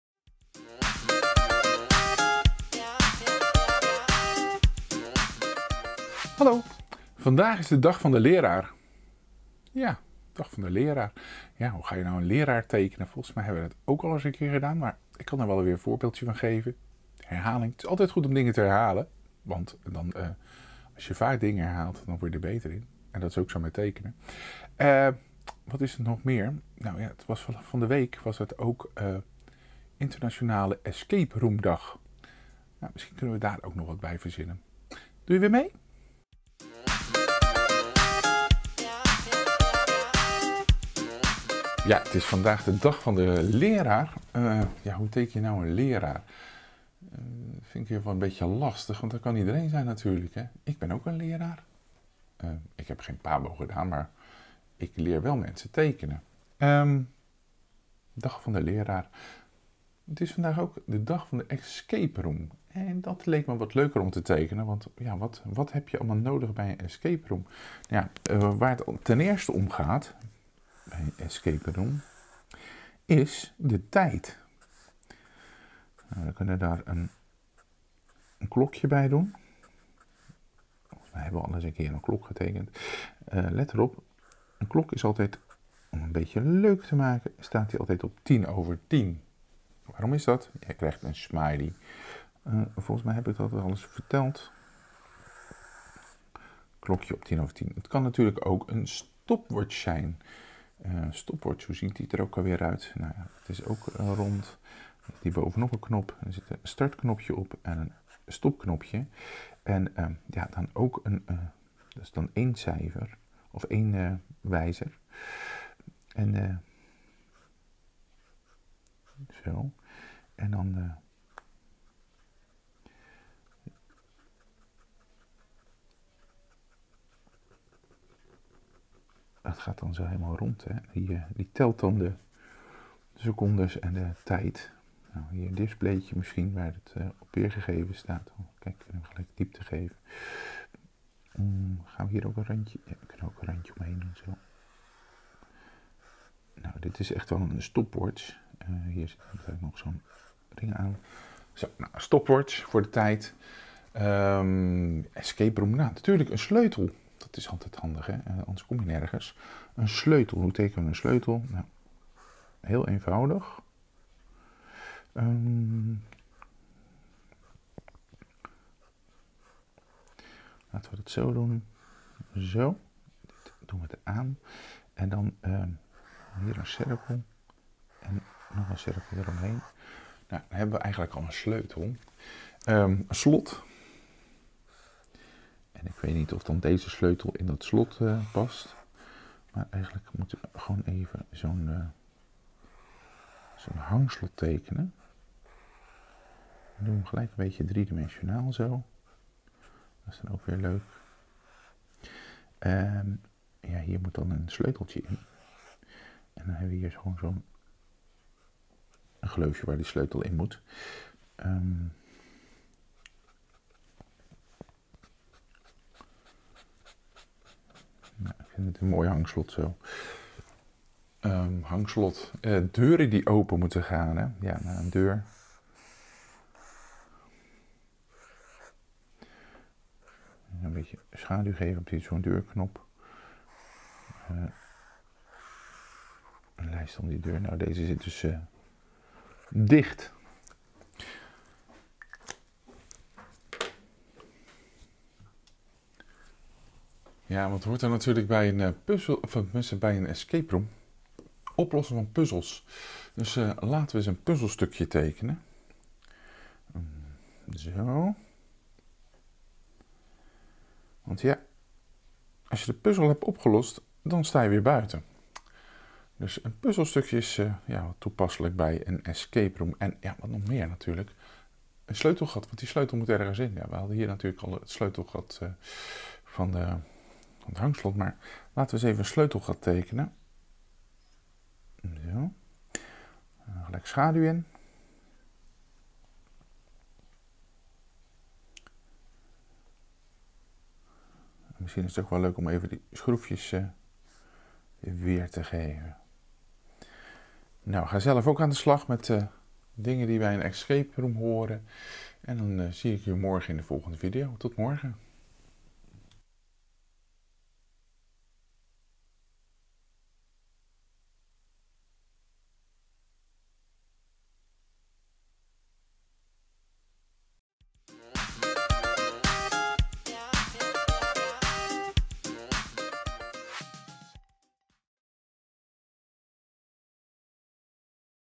5 oktober - Dag van de Leraar of Escape room dag - Hoe teken ik dat?
In 2022 maak ik elke dag een tekening volgens de mediakalender. Teken je mee?